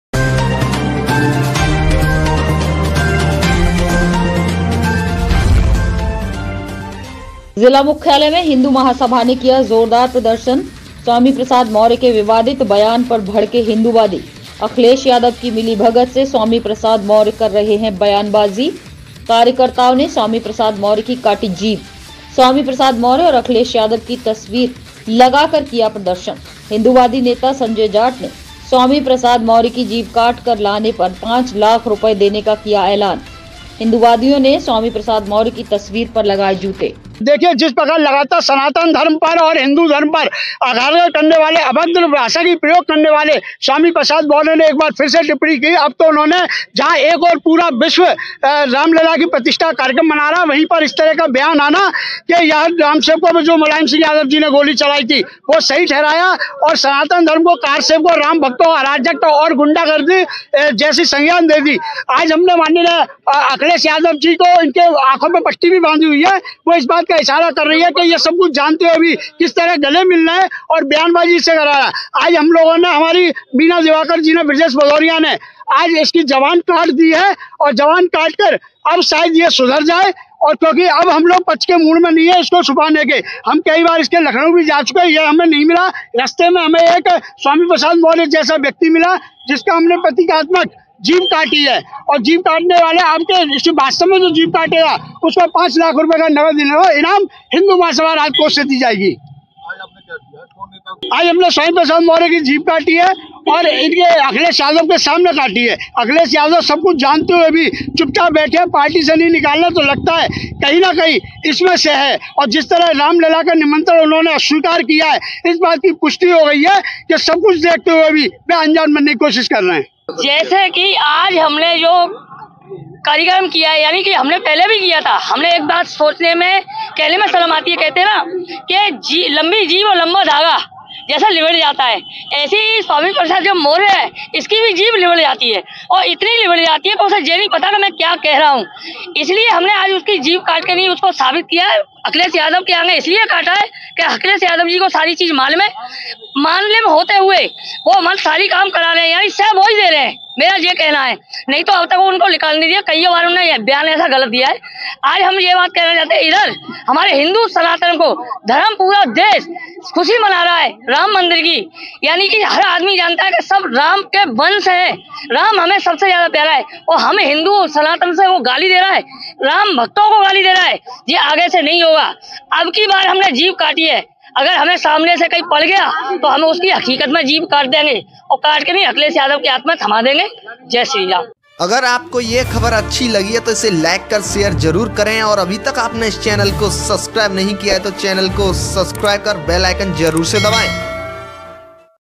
हिंदूवादी नेताओं ने स्वामी प्रसाद मौर्य की काटी जीभ | Agra News | IN 24
हिंदूवादी नेताओं ने स्वामी प्रसाद मौर्या की काटी जीभ |

Only Whatsapp - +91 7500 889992

डिस्क्लेमर - वीडियो में दिखाई गई राय लोगो की अपनी राय है,लोगो के विचारों से IN_24 यूट्यूबचैनल या शो का रिपोर्टर सहमत या असहमत नहीं हैं l